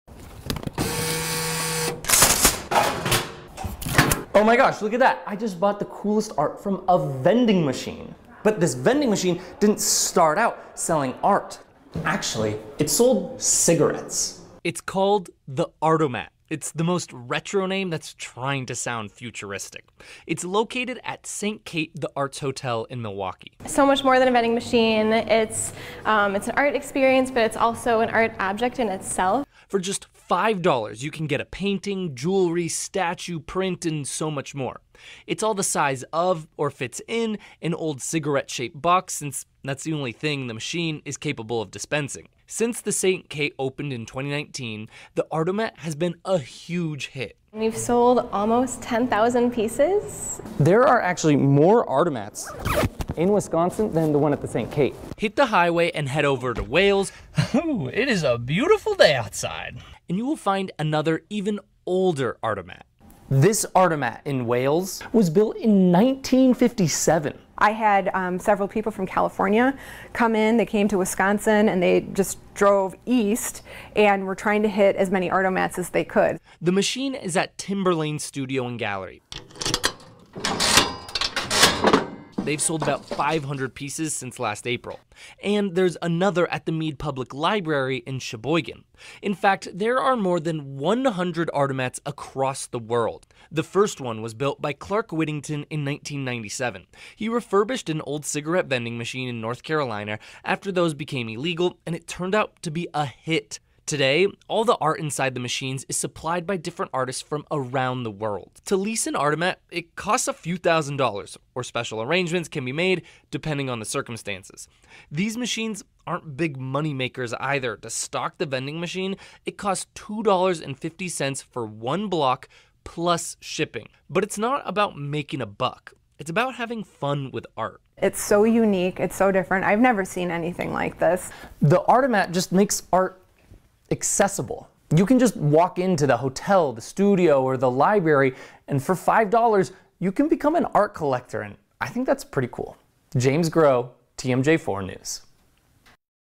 Vintage cigarette vending machine turned $5 art dispenser
What if you took something standard, like a vending machine, and made it really cool?  That's not enough, though. Let's take an old piece of machinery that essentially became illegal and give it a new purpose. Introducing, the Art-o-mat.

It's a retired cigarette vending machine that dispenses art. For just $5, you can get a print, painting, jewelry, statue, and so much more. There are more than 100 across the world, and there just so happens to be three in Wisconsin.